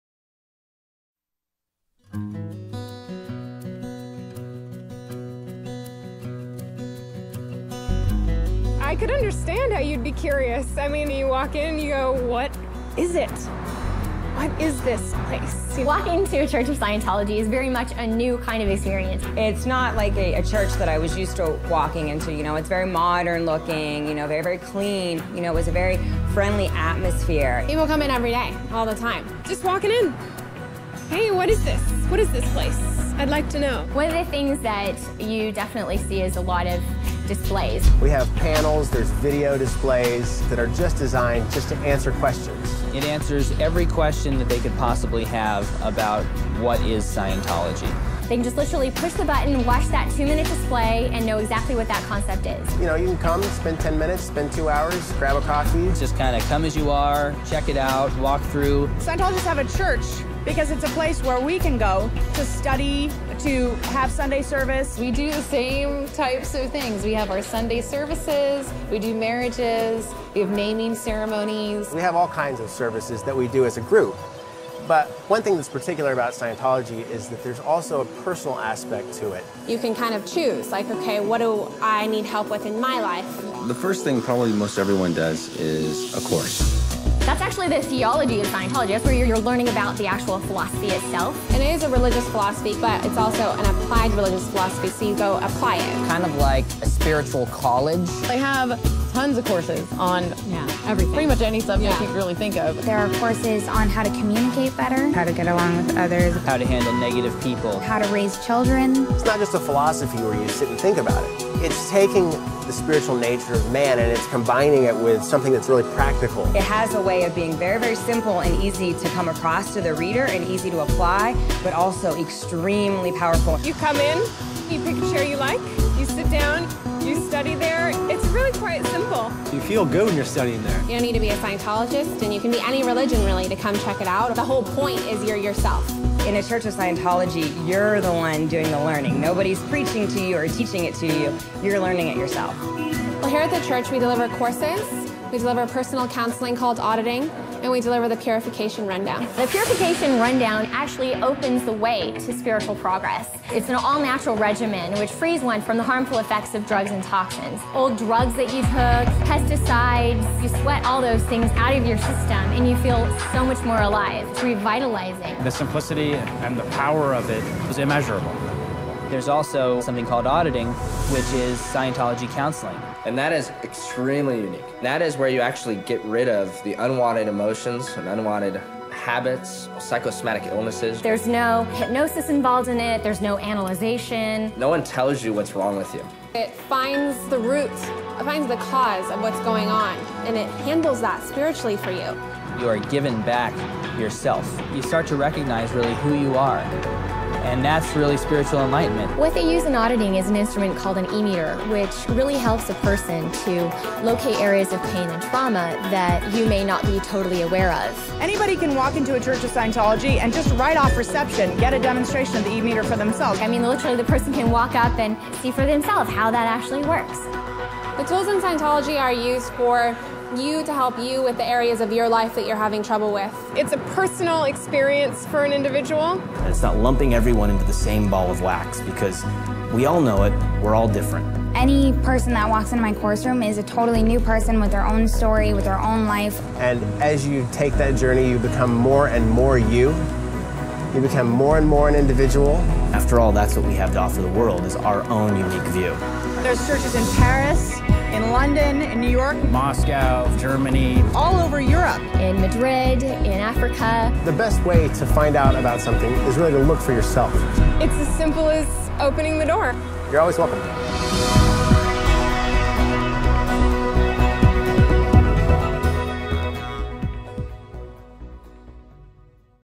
Im Inneren einer Scientology Kirche | Urteilen Sie selbst
Es gibt über 10.000 Scientology Kirchen, Missionen und Gruppen auf der Welt. "Inseln der spirituellen Freiheit" oder "gesitig-kulturelle Ruhepunkte" sind treffende Beschreibungen. Waren Sie schon mal in einer Scientology Kirche? Es gibt regelmäßig Tage der offenen Tür z.B. in der Scientology Kirche Hamburg, Berlin, München, Stuttgart, Düsseldorf, Frankfurt, etc.
Es lohnt sich. Es ist eine besondere Erfahrung. Dass Scientology eine Sekte ist, widerlegt sich ziemlich schnell von selbst. Unternehmen Sie mal eine Reise ins Innere der Scientology und besuchen Sie eine Kirche, schauen Sie selbst, urteilen Sie selbst und unvoreingenommen. Danke!